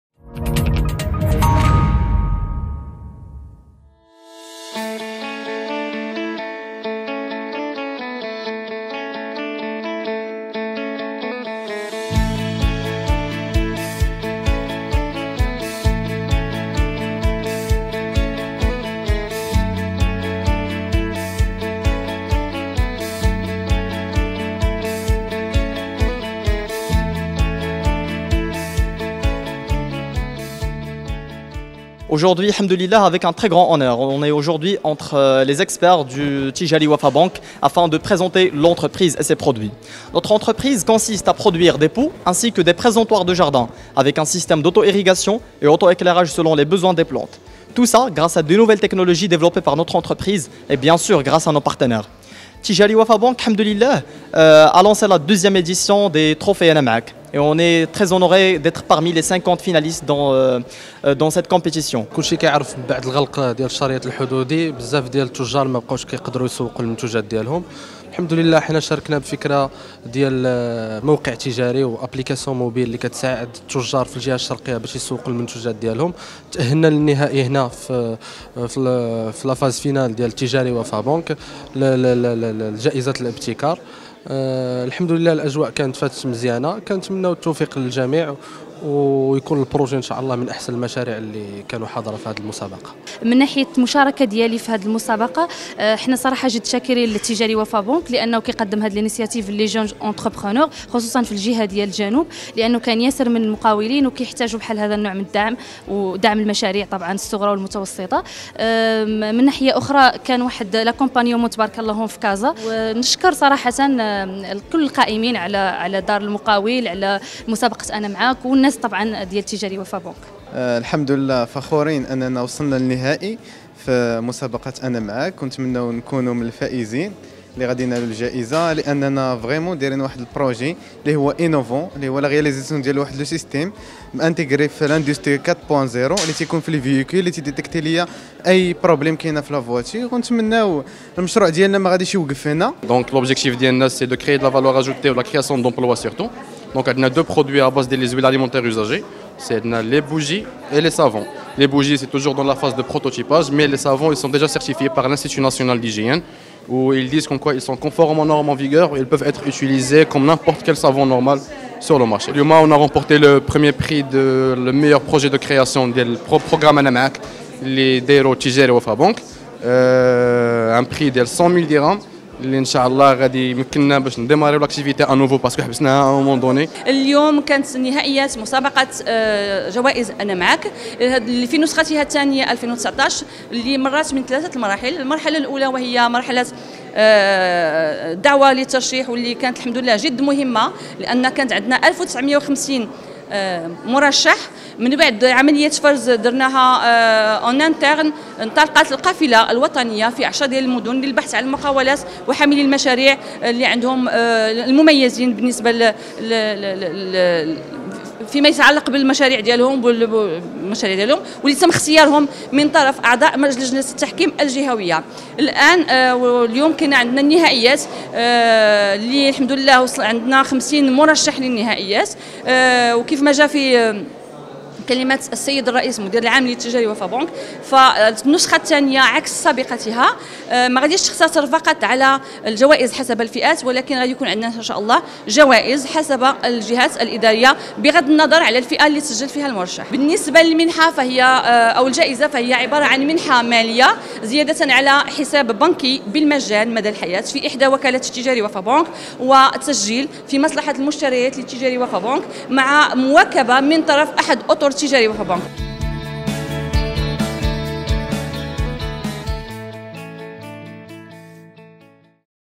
Trophées Ana Maâk : Le Groupe Attijariwafa Bank récompense 15 porteurs de projets
Trophées Ana Maâk